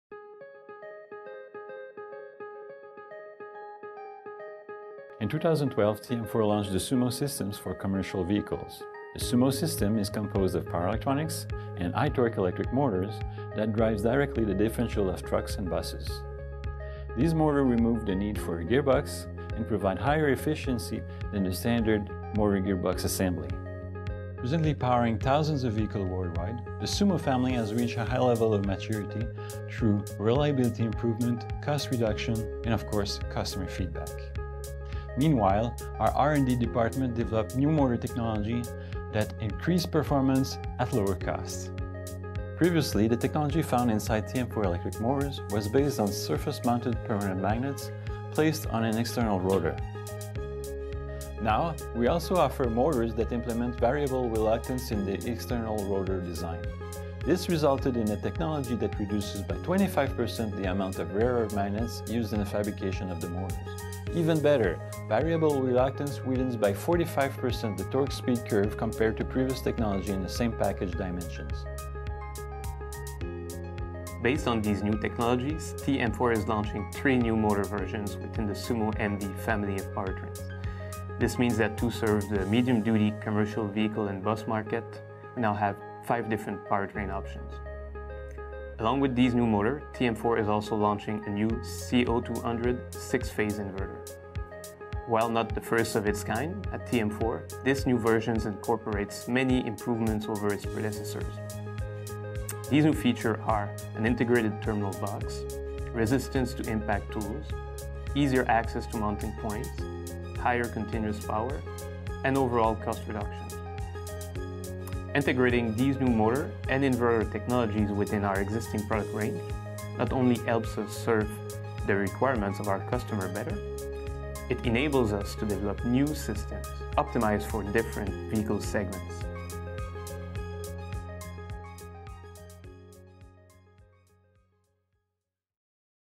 TM4 - Get more torque with the new SUMO MD
TM4 will launch at the 29th Electric Vehicle Symposium & Exhibition (EVS29) three new SUMO MD powertrain declination offering an increase of up to 45% in torque and speed, thanks to a new technological advancement.